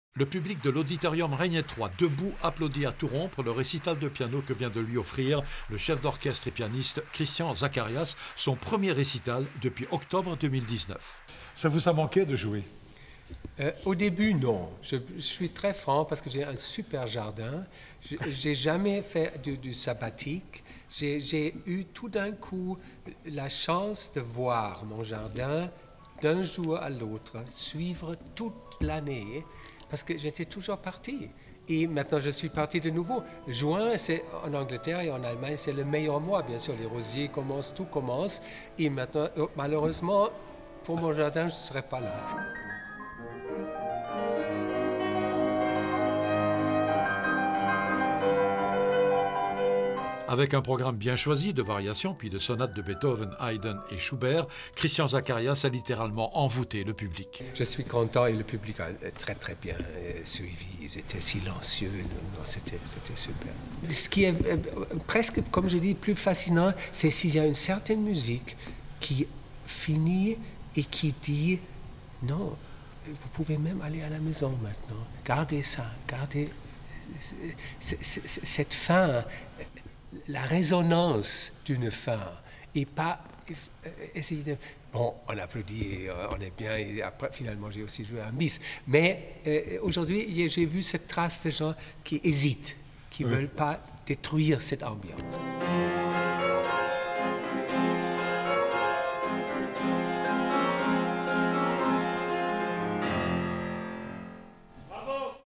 Christian Zacharias au sommet de son art
Après avoir dirigé l' Orchestre Philharmonique de Monte-Carlo le 14 février dernier, le pianiste Christian Zacharias était de retour pour un récital de piano à l'auditorium.

Un reportage musical signé José Sacré